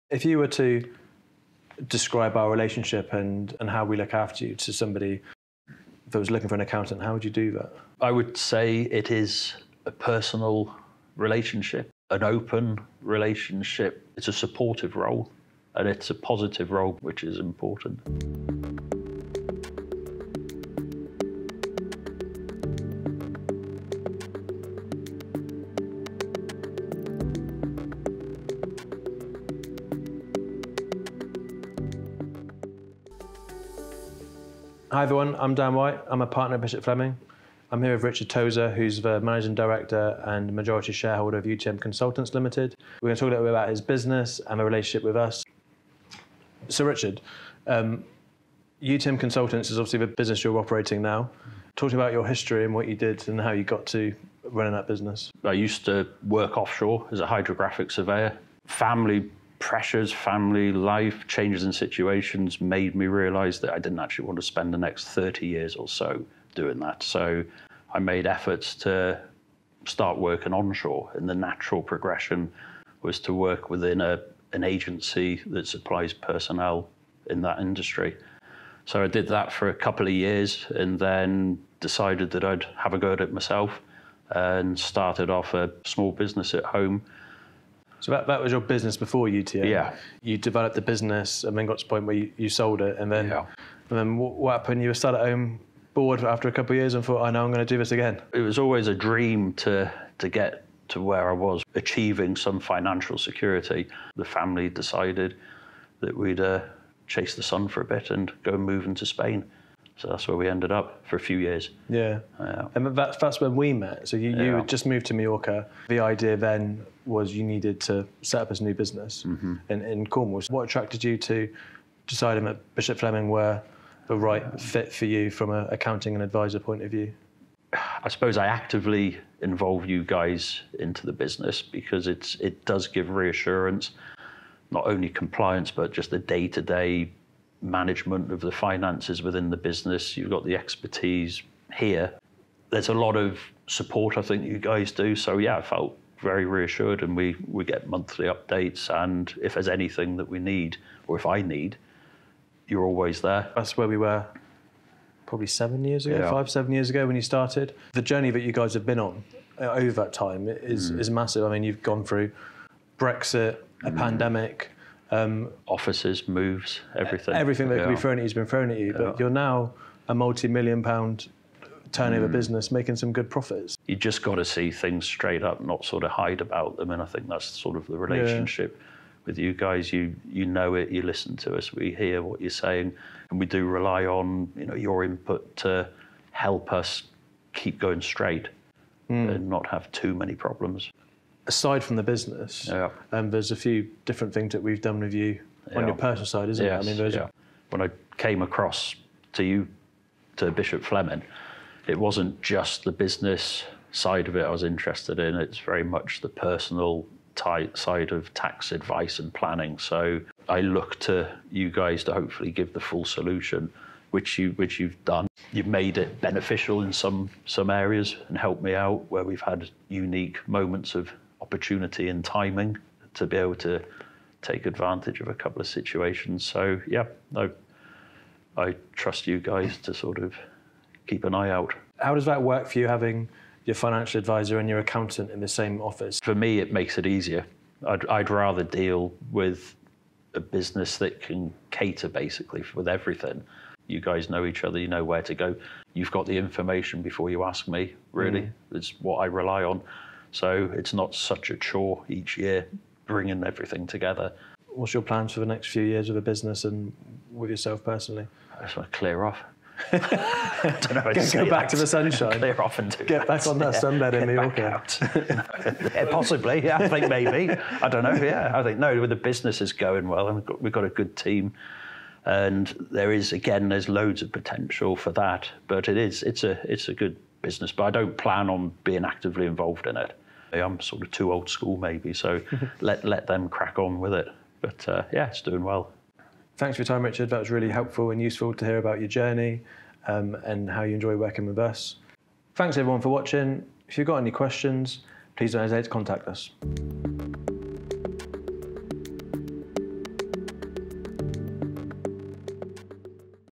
Technology, Innovation & Growth | Conversations with UTM Consultants | Bishop Fleming
For many years, UTM Consultants have been working with Bishop Fleming as they have progressed within the technology sector.

Managing Director Richard Tozer discusses how Bishop Fleming has helped guide his finances and the development of a personal relationship he has built with the company.

UTM Consultants provide bespoke personnel solutions to global offshore services companies and work closely with the very best technical freelance consultants in the industry.